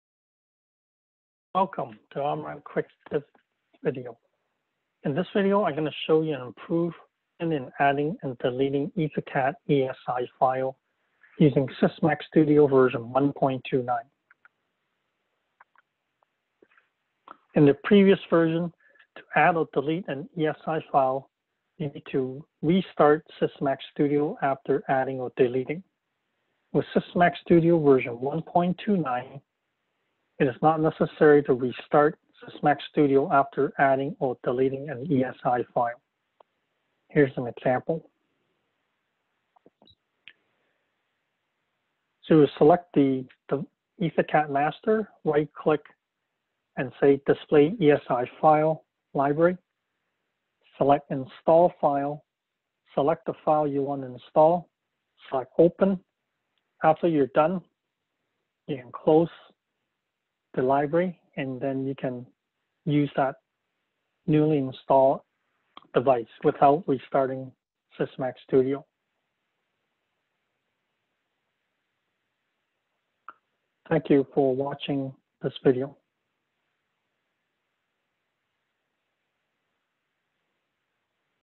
OMRON Sysmac Studio EtherCAT ESI File
Sysmac Studio V1.29 EtherCAT ESI file (version 1.29 or higher required)

Improvement in adding and deleting EtherCAT’s ESI files to Sysmac Studio library.